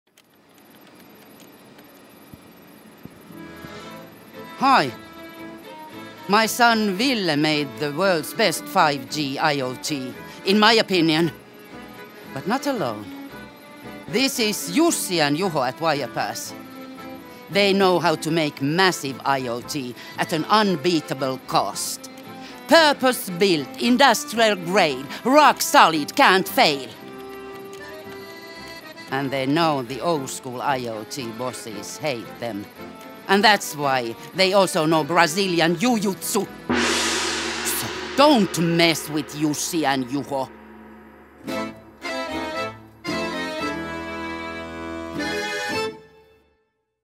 Straight Outta Tampere - Episode 3 - Don’t mess with Jussi and Juho.
Jussi and Juho may not look mean. But boy, are they mean. And they hate bad IoT. They are part of the team that invented the world’s first non-cellular 5G IoT. It’s faster, better, cheaper. It makes old IoT look ancient. Like something for the Flintstone family. But Jussi and Juho also know other stuff. Martial art stuff. Yes, so please, don’t mess with them. It can go very, very wrong.